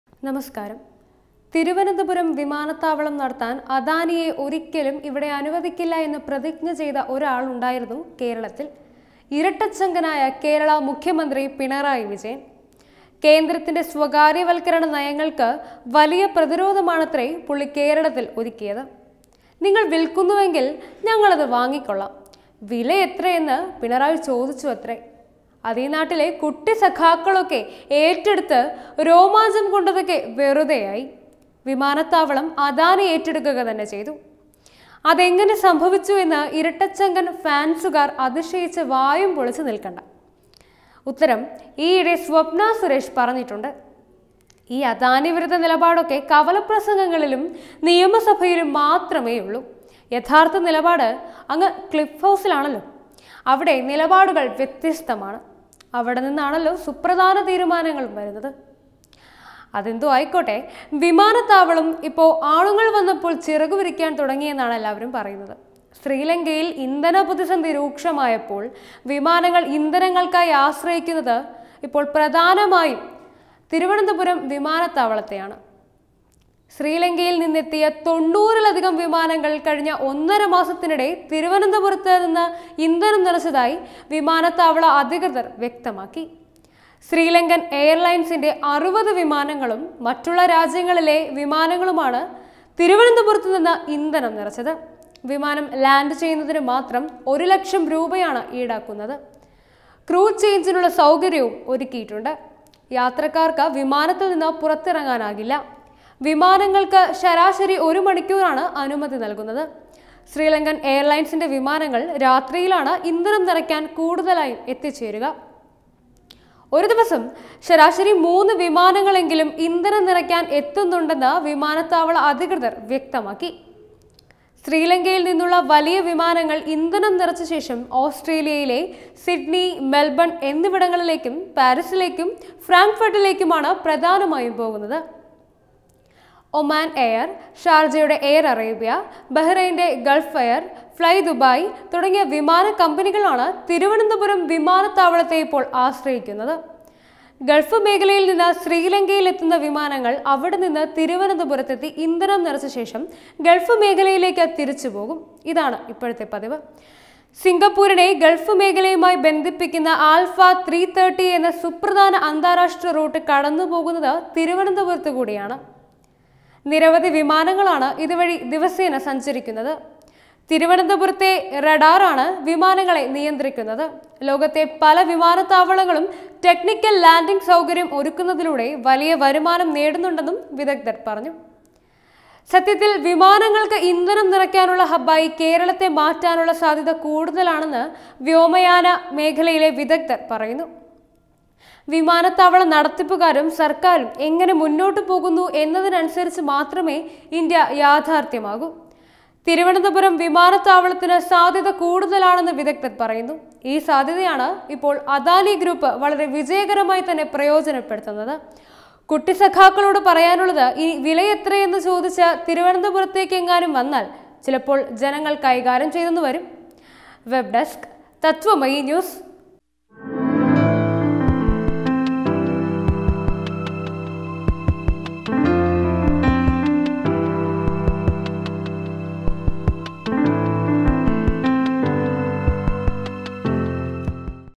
വിമാനത്താവളത്തിന്റെ വിലയെത്രയെന്നു ചോദിച്ച കുട്ടി സഖാവിനെ കണ്ടവരുണ്ടോ ? | Thiruvananthapuram Airport
വിമാനത്താവളം നടത്താൻ അദാനിയെ അനുവദിക്കില്ലെന്ന് പറഞ്ഞ ഇരട്ട ചങ്കനെ കണ്ടവരുണ്ടോ?  | Thiruvananthapuram Airport